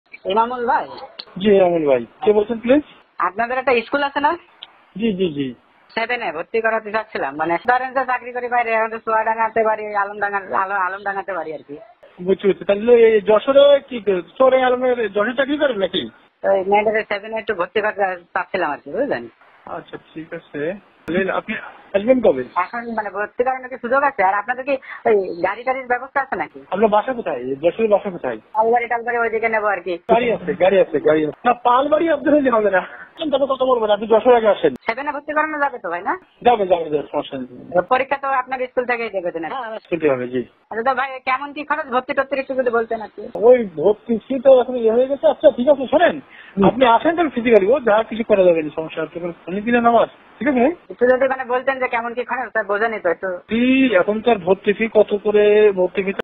শংকরপুরে বারোমাসী স্কুলে অর্থ বাণিজ্যের অডিও ফাঁস
যশোর শহরের নাজির শংকরপুরে খোঁজ মিলেছে একটি বারোমাসী স্কুলের। স্কুলটির নাম গোল্ডেন লাইফ প্রিক্যাডেট। এখানে বছরভর চলে অবৈধ ভর্তি ও নিয়োগ বাণিজ্য কার্যক্রম। যার একটি অডিও ক্লিপ আমাদের হস্তগত হয়েছে। অডিওতে কথিত স্কুলটির পরিচালক ইনামুল কবীর ও একজন অভিভাবকের কথোপকথনে বছরভর ভর্তি অভিযোগের সত্যতা মিলেছে।